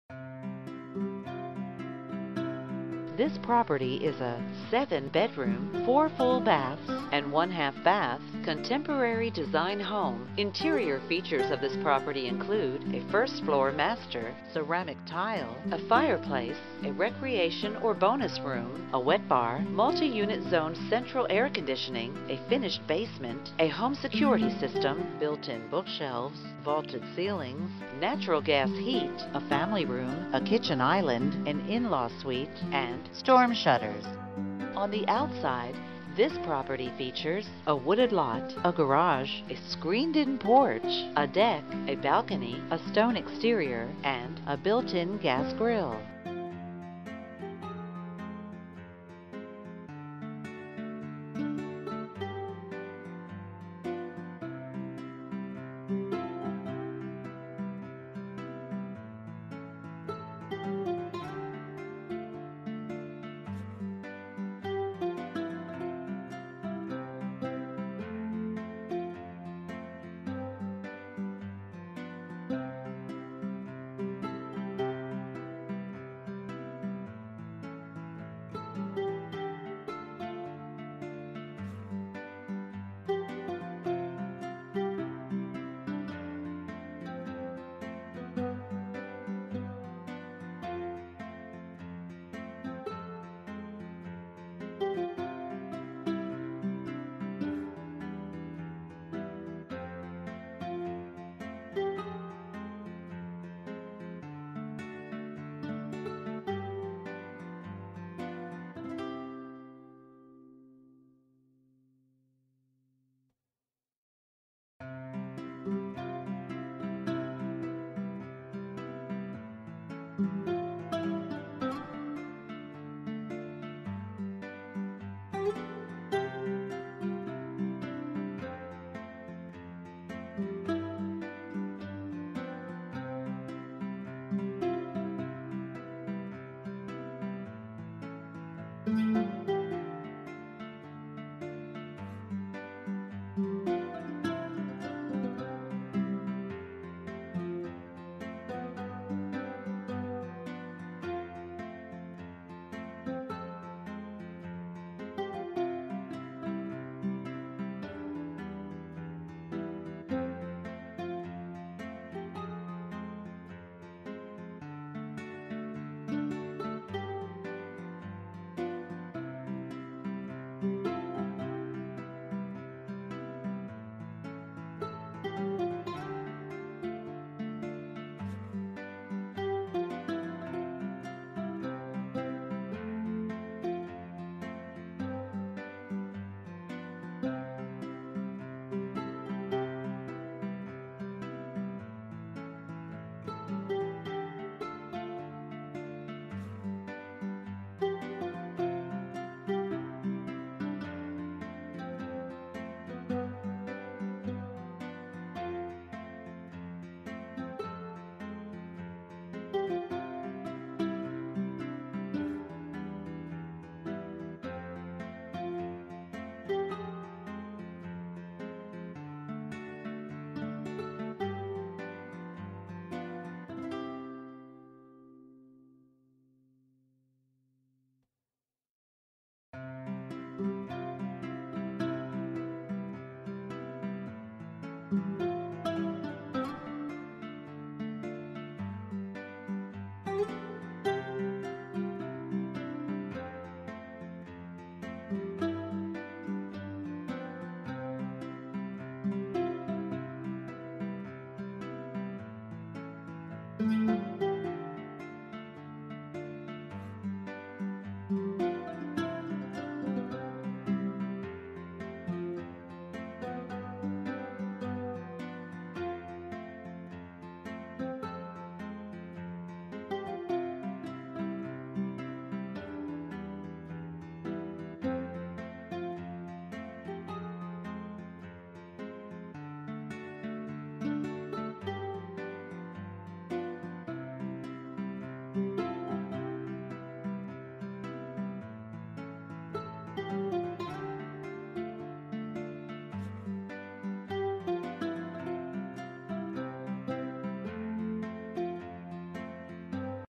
4340 Chatuge Drive, Buford, GA, 30519 Tour - $560,000
Represented by: Joseph Stockdale
Solid Source Realty GA,(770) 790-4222.
7 Bedrooms - 4 Full Baths, 1 Half Bath
This is a ONE OF A KIND   must see! The only home in the neighborhood with this much acreage and still has community amenities with easy access to significant roadways!  Right away, you will see all the love poured into this home by way of the beautiful   longlasting / high quality   functional detail.  This once 1998 looking home is  decked out  in over $227K custom upgrades that will be MOVE IN READY.  Custom solid wood cabinets, flooring, paint, lighting, stainless steal appliance package and completely finished basement.